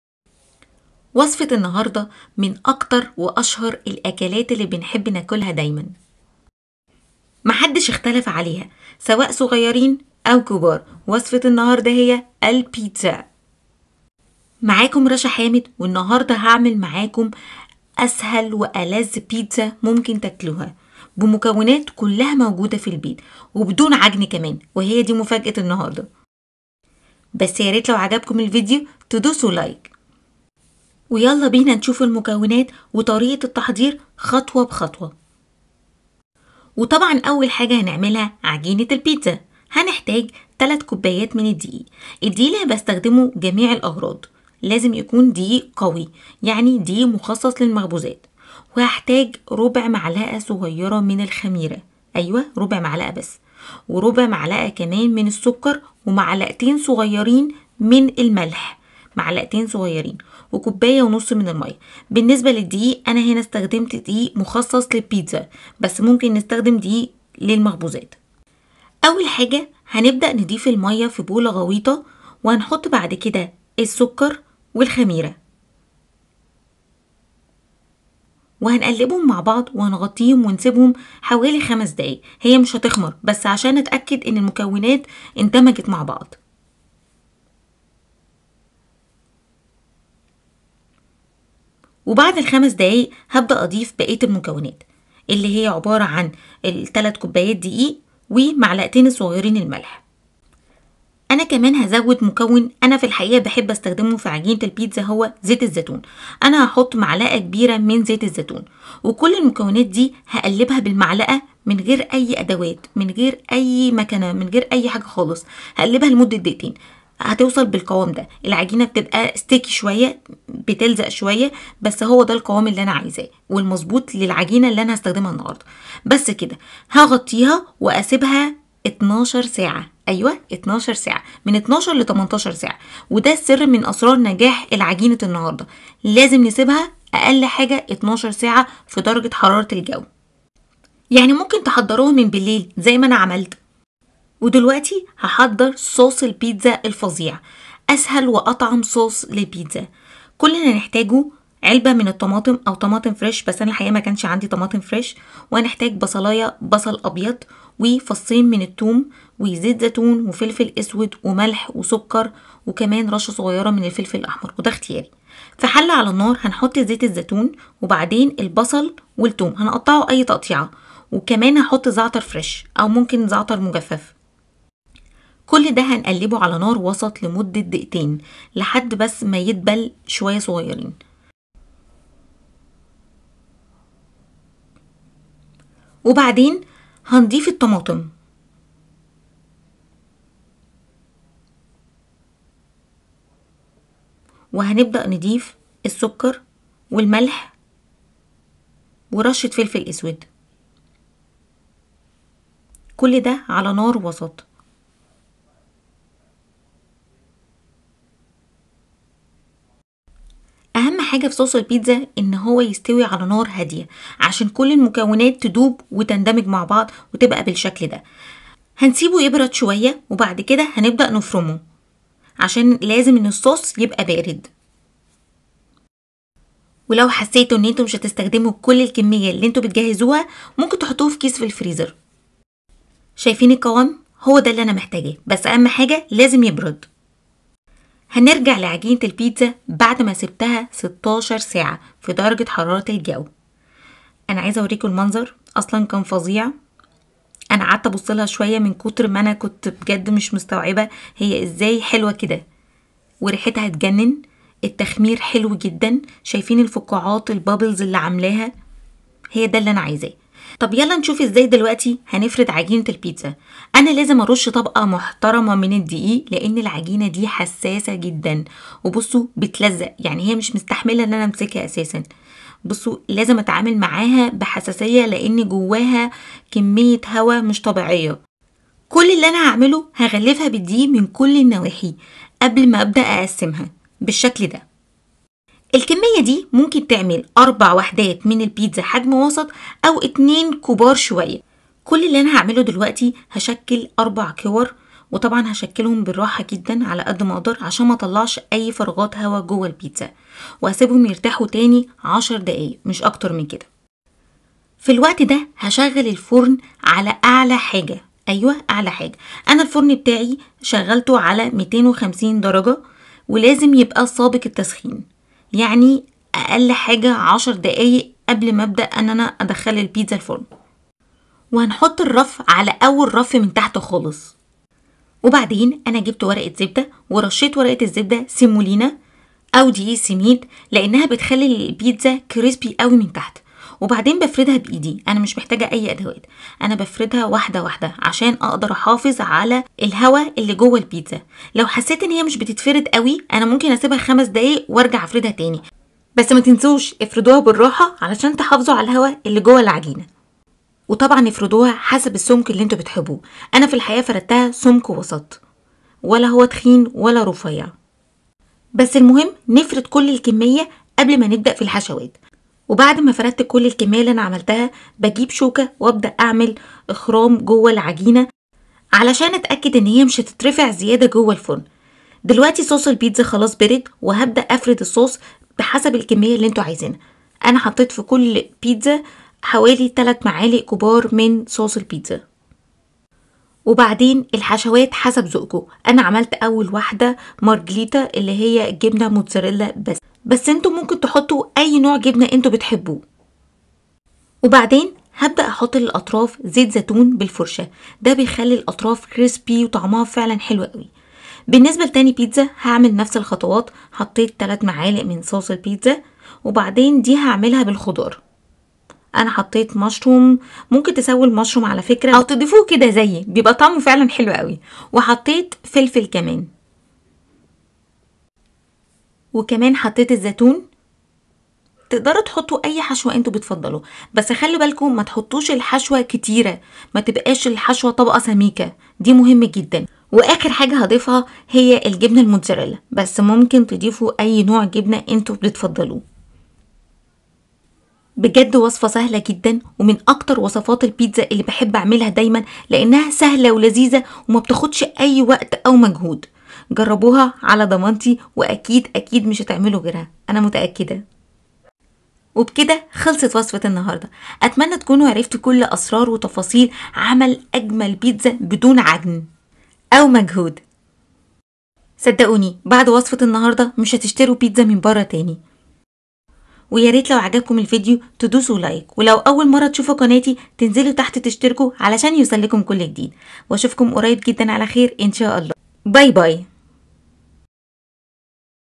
🍕😋طريقة عمل ألذ بيتزا بدون عجن والطعم يجنن
المكونات
٣ كوب من الدقيق متعدد الاغراض ٤٠٠ جرام
ربع معلقة صغيرة خميرة ١ جرام
ربع معلقة صغيرة سكر ١ جرام
عدد ٢ معلقة صغيرة ملح ١٢ جرام
كوب ونصف من الماء ٣٥٠ جرام
صوص البيتزا
علبة من صوص الطماطم او حوالي ٤ طماطم فريش مسلوقة ومن غير القشرة الخارجية